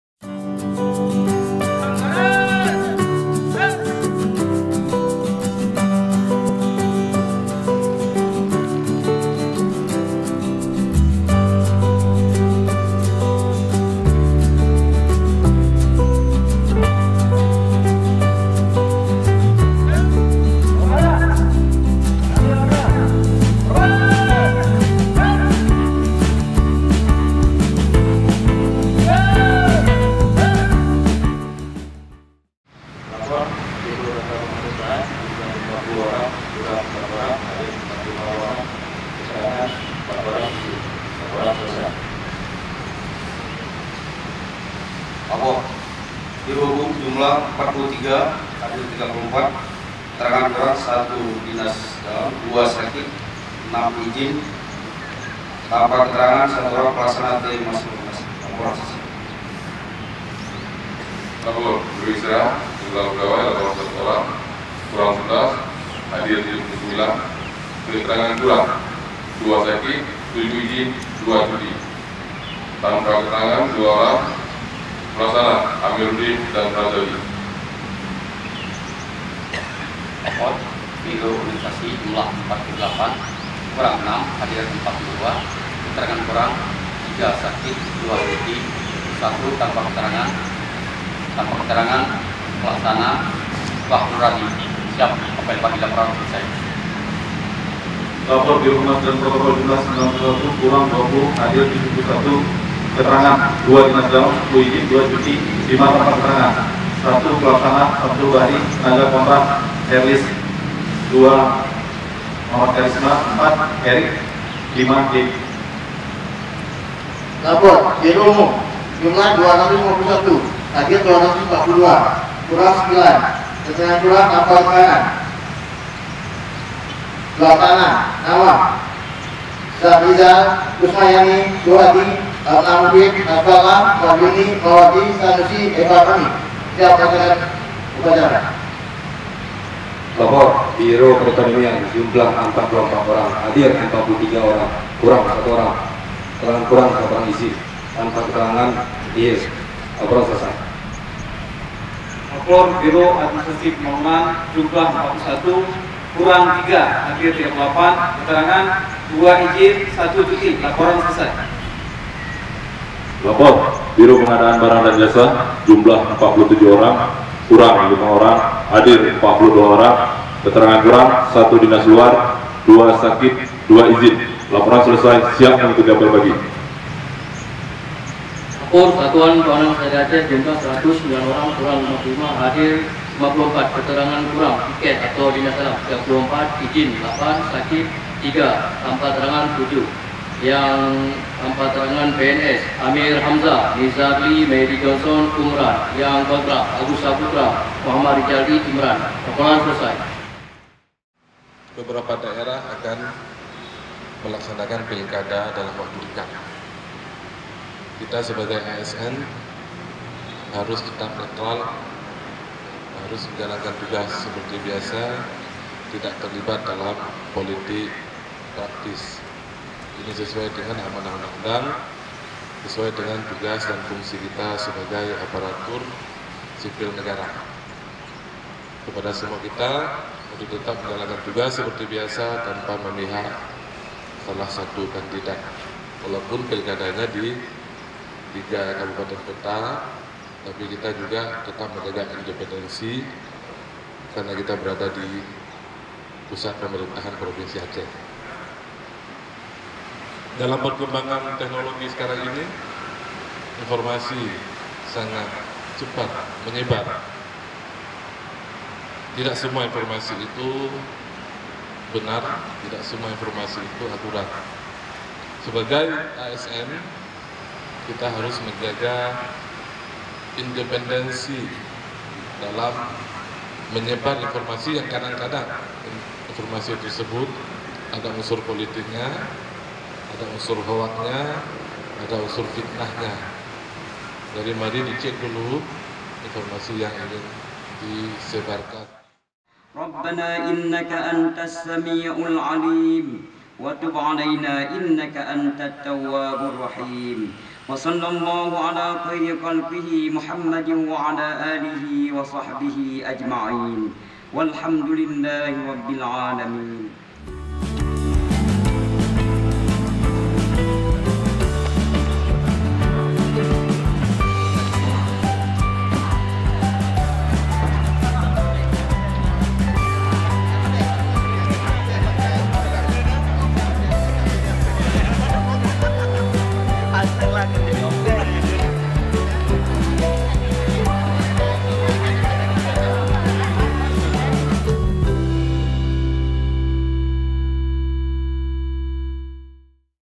ASN Diminta tidak Berpolitik
Banda Aceh - Kepala Biro Humas dan Protokol Setda Aceh, Mulyadi Nurdin, meminta Aparatur Sipil Negara untuk tidak terlibat dalam aktifitas politik. Hal tersebut disampaikan karena dalam waktu dekat Pilkada Bupati dan Wali Kota akan digelar di beberapa Kabupaten dan Kota di Aceh.

"Kita berada di pusat pemerintahan. Tetap jalankan tugas dan jangan terlibat dalam aktifitas politik," kata Mulyadi saat memimpin apel pagi, Senin 05/03/2018.